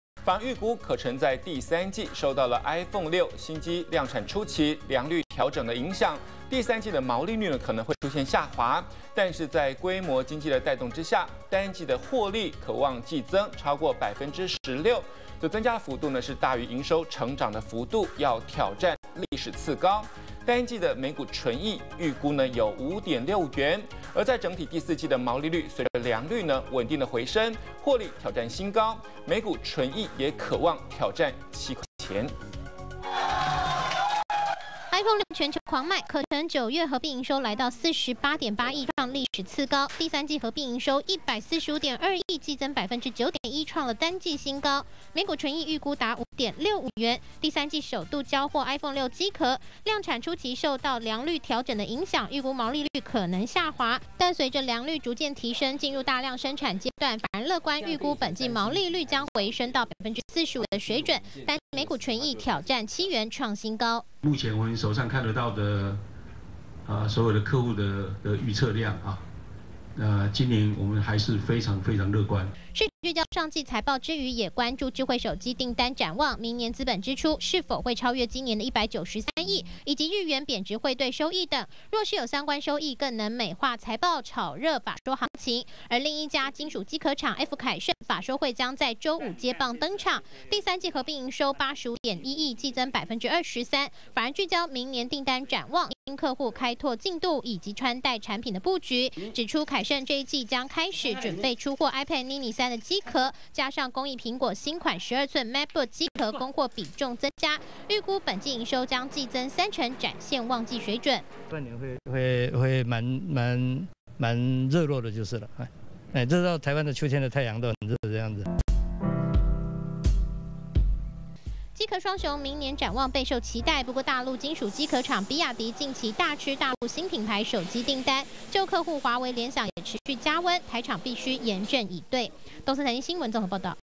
機殼廠鍍金 可成年底大啃蘋果單
經濟日報 可成法說會 明天登場，法人預估，可成 第三季 受到 iPhone 6新機 量產初期 良率調整影響，毛利率 可能下滑，但是 在規模經濟帶動下，單季獲利 可望季增 超過16%，增幅大於 營收成長的幅度，挑戰歷史次高，單季每股純益 預估 5.65元；第四季毛利率 隨著良率穩定回升，獲利 挑戰新高，每股純益 挑戰7元。